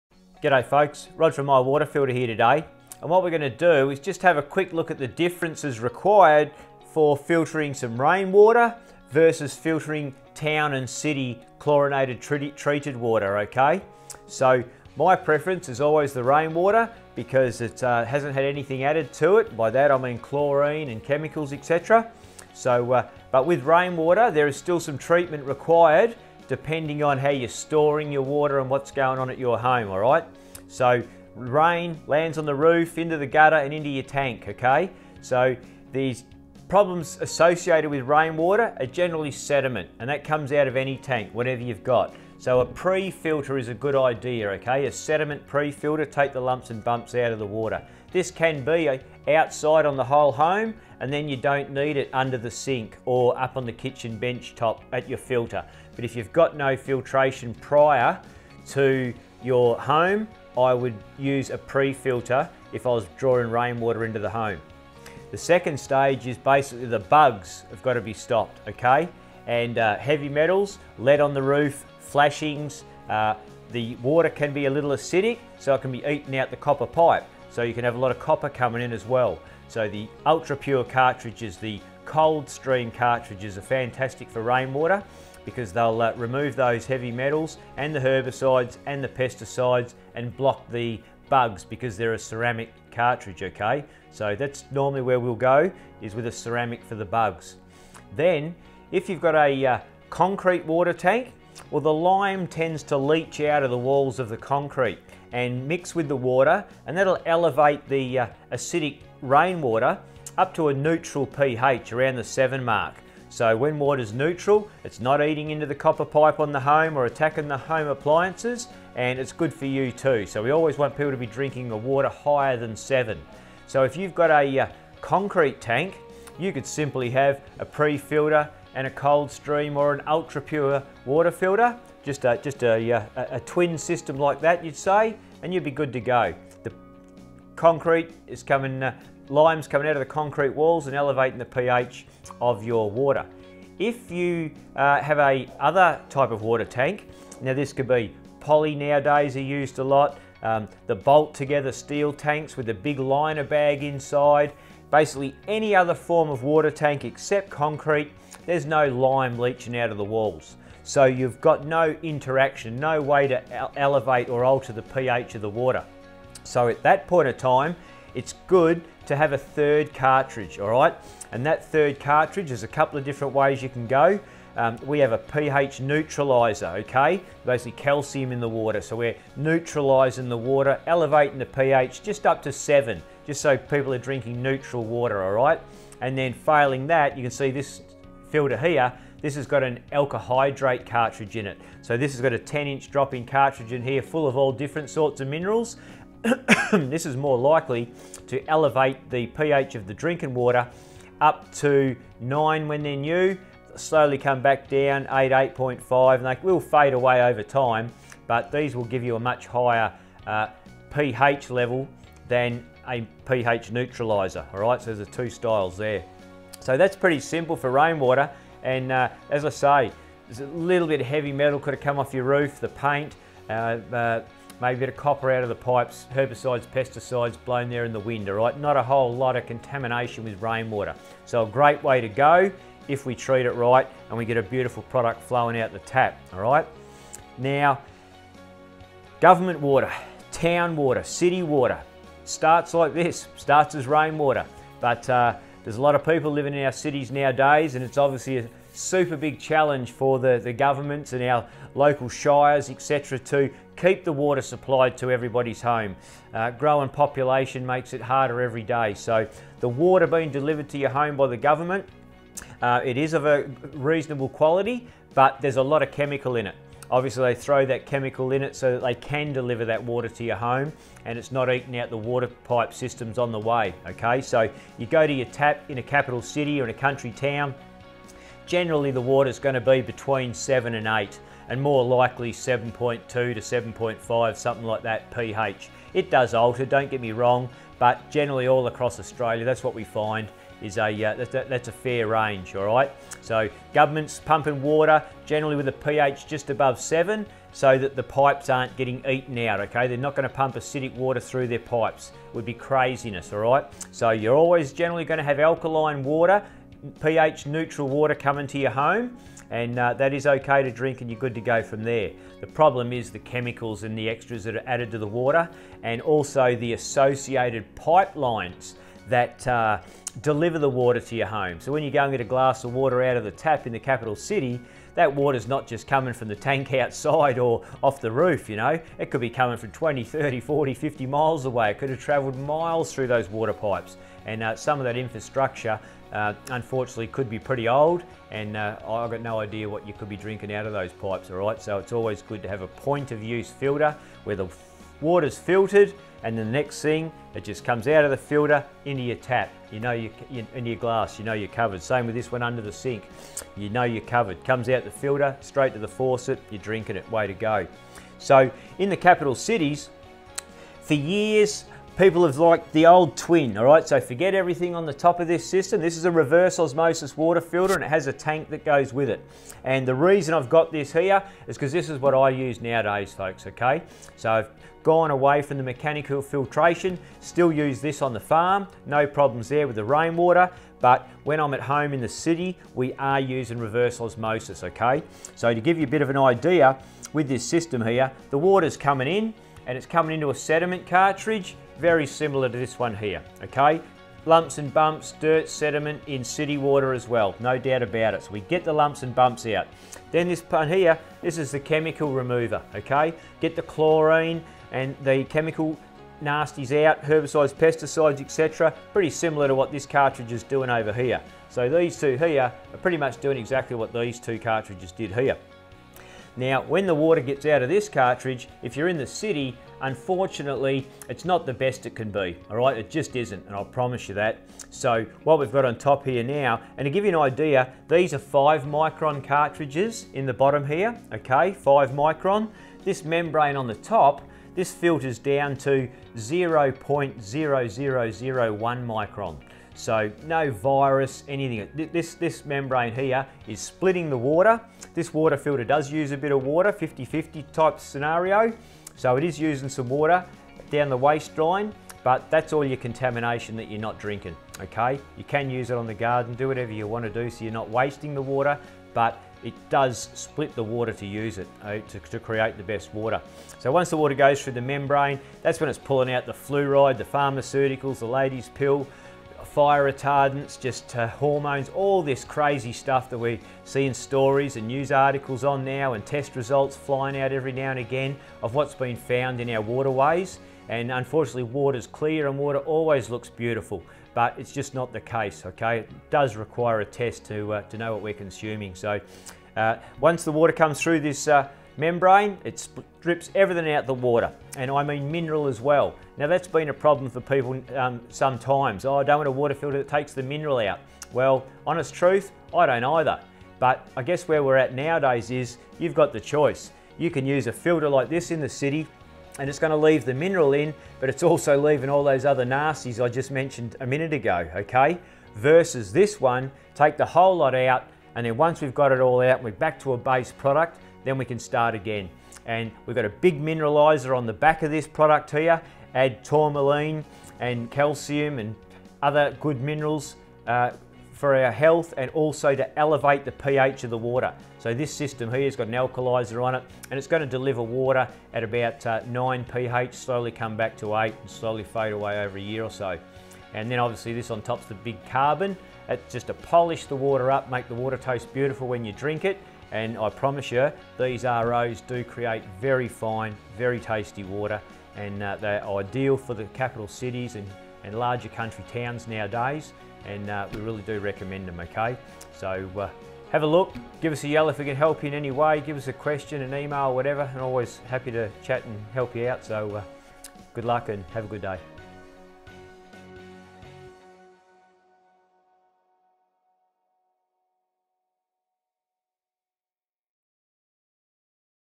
Differences between filtering Rainwater vs Town and City Water
Hi and welcome to our video on Differences between filtering Rainwater vs Town and City Water. To learn more, please click the link above

Transcription:
G'day folks. Rod from My Water Filter here today, and what we're gonna do is just have a quick look at the differences required for filtering some rainwater versus filtering town and city chlorinated, treated water, okay? So my preference is always the rainwater, because it hasn't had anything added to it. By that, I mean chlorine and chemicals, et cetera, but with rainwater, there's still some treatment required, depending on how you're storing your water and what's going on at your home, alright? So rain lands on the roof, into the gutter, and into your tank, okay?

So these problems associated with rainwater are generally sediment, and that comes out of any tank, whatever you've got. So a prefilter is a good idea, okay? A sediment prefilter will take the lumps and bumps out of the water. This can be outside on the whole home, and then you don't need it under the sink or up on the kitchen benchtop at your filter.

But if you've got no filtration prior to your home, I would use a prefilter if I was drawing rainwater into the home. The second stage is basically the bugs have gotta be stopped, okay? And heavy metals, lead on the roof, flashings, the water can be a little acidic, so it can be eating out the copper pipe, so you can have a lot of copper coming in as well.

So the ultra-pure cartridges, the Coldstream cartridges are fantastic for rainwater, because they'll remove those heavy metals and the herbicides and the pesticides and block the bugs, because they're a ceramic cartridge, okay? So that's normally where we'll go is with a ceramic for the bugs. Then, if you've got a concrete water tank, well the lime tends to leach out of the walls of the concrete and mix with the water, and that will elevate the acidic rainwater up to a neutral pH around the seven mark, so when water's neutral, it's not eating into the copper pipe on the home or attacking the home appliances, and it's good for you, too.

So we always want people to be drinking the water higher than seven, so if you've got a concrete tank, you could simply have a prefilter and a Coldstream or an ultra-pure water filter, just a twin system like that, you'd say, and you'll be good to go. Lime's coming out of the concrete walls and elevating the pH of your water. If you have a other type of water tank, now this could be poly nowadays are used a lot, the bolt-together steel tanks with the big liner bag inside, basically any other form of water tank except concrete, there's no lime leaching out of the walls, so you've got no interaction, no way to elevate or alter the pH of the water. So at that point in time, it's good to have a third cartridge, alright? And that third cartridge, there's a couple of different ways you can go.

We have a pH neutralizer, okay? Mostly calcium in the water, so we're neutralizing the water, elevating the pH just up to seven, just so people are drinking neutral water, alright? And then following that, you can see this filter here, this has got an alkahydrate cartridge in it. So this got a 10-inch drop-in cartridge in here, full of all different sorts of minerals. This is more likely to elevate the pH of the drinking water up to nine when they're new, slowly come back down eight, 8.5, and that will fade away over time, but these will give you a much higher pH-level than a pH neutralizer, alright? So there's two styles there. So that's pretty simple for rainwater, and as I say, there's a little bit of heavy metal could've come off your roof, the paint, maybe the copper out of the pipes, herbicides, pesticides blowing there in the wind, alright? Not a whole lot of contamination with rain water, so a great way to go, if we treat it right, and we get a beautiful product flowing out the tap, alright?

Now government water, town water, city water starts like this, starts as rainwater, but there's a lot of people living in our cities nowadays, and it's obviously a super big challenge for the government and our local shires, et cetera to keep the water supply to everybody's home. A growing population makes it harder every day, so the water being delivered to your home by the government, it is of a reasonable quality, but there's a lot of chemical in it.